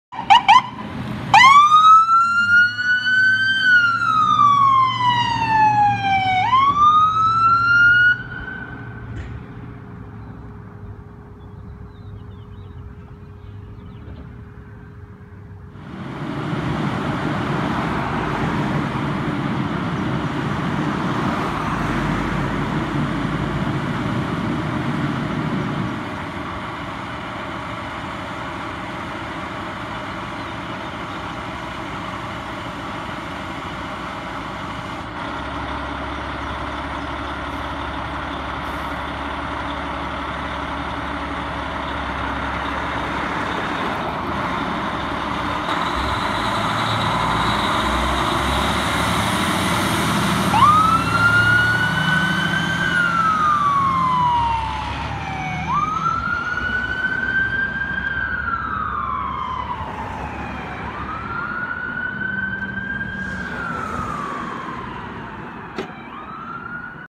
Queensbury Central Fire Utility 115 and *NEW* Engine Rescue 316 Responding
Utility 115 is a 2015 Ford F550, and ER 316 is a 2017 Pierce Enforcer, Responding to a MVA in this video.